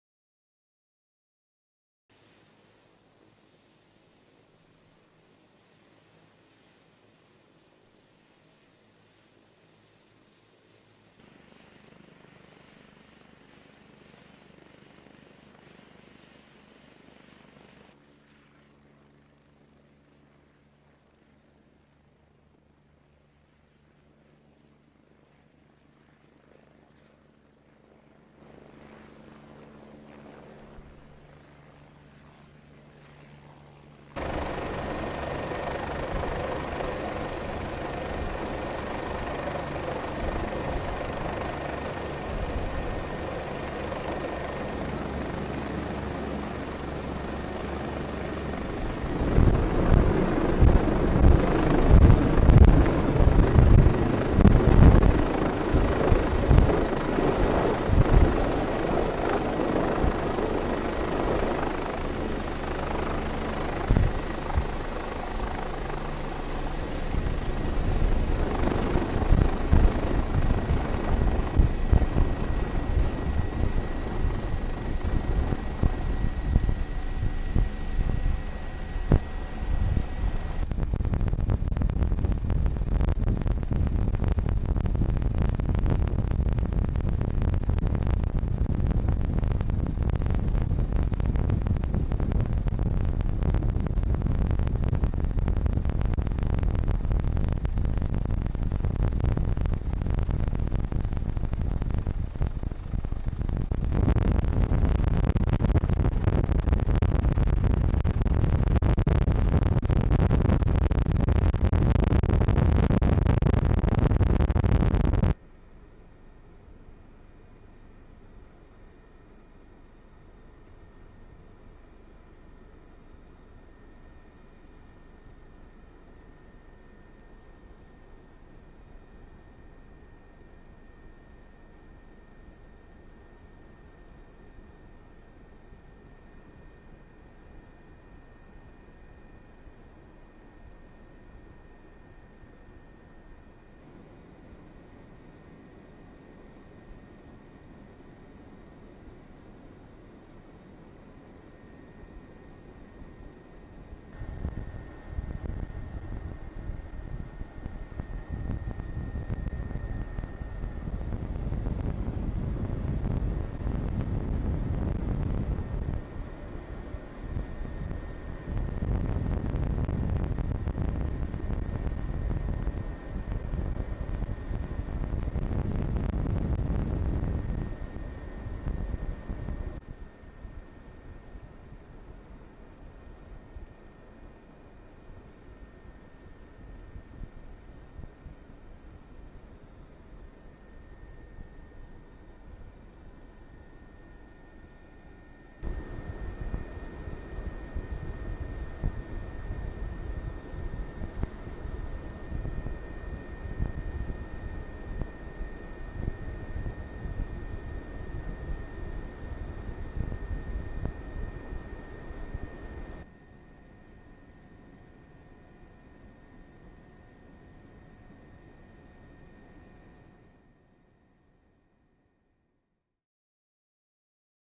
U.S ARMY STRONG - UH 72A Lakota Helicopter • Cockpit & Aerial View 2016
U.S ARMY STRONG - UH 72A Lakota Helicopter • Cockpit & Aerial View 2016 #1